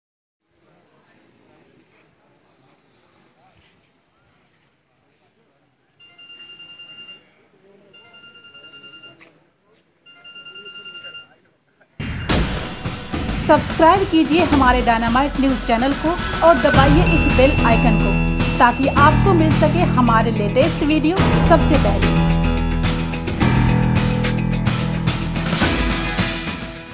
आजमगढ़: मोटर साइकिल की टक्कर से दो व्यवसायियों की मौत व एक की हालत गंभीर
आजमगढ़: बिलरियागंज थाने में मोटर साइकिल की टक्कर से दो व्यवसायियों की मौत व एक की हालत गंभीर, पोस्टमार्टम हाउस के बाहर मौजूद मृतकों के परिजन
Full Story: